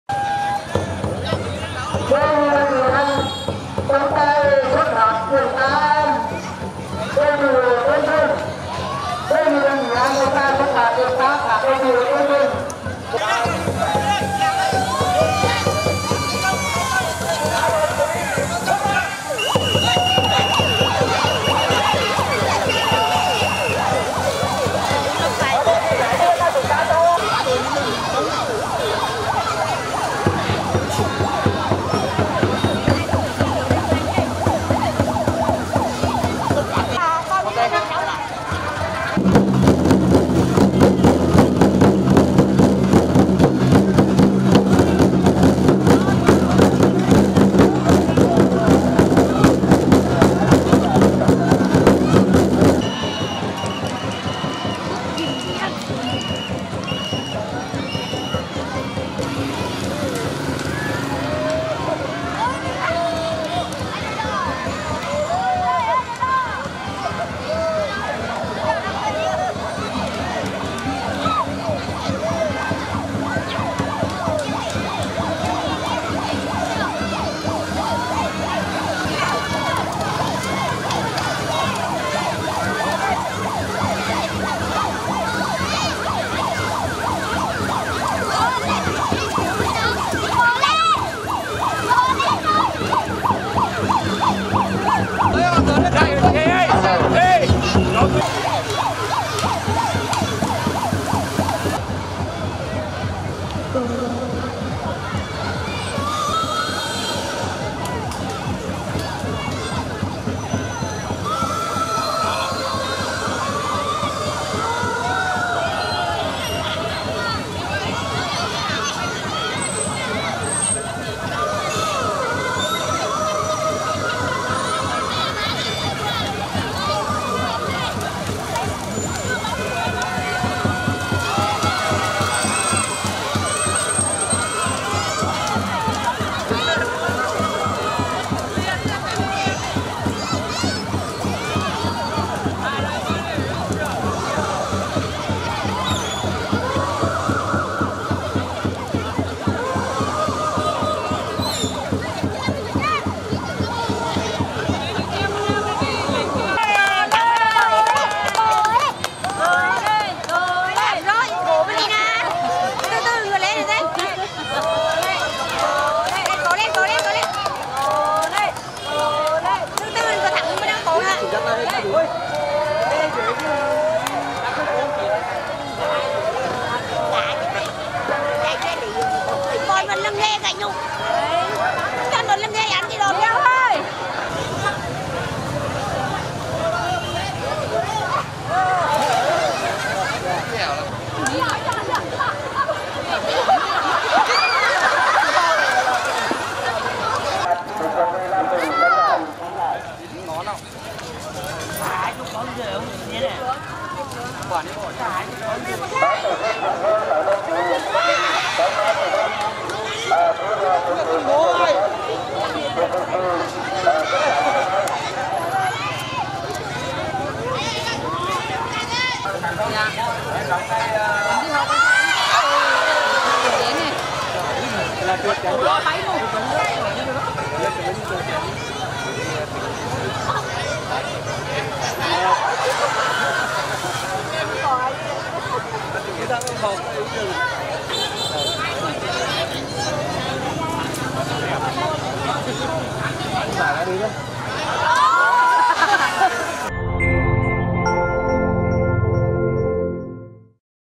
Hà Tĩnh: Đua thuyền trên sông Cụt mừng ngày đại đoàn kết toàn dân 2018
Chiều 11/11, tổ dân phố 10, phường Tân Giang (TP Hà Tĩnh) nô nức Lễ hội đua thuyền lần thứ 9 thiết thực chào mừng 88 năm ngày thành lập Mặt trận dân tộc thống nhất Việt Nam và ngày hội Đại đoàn kết toàn dân.

Báo Hà Tĩnh - Tin tức Hà Tĩnh cập nhật 24h liên tục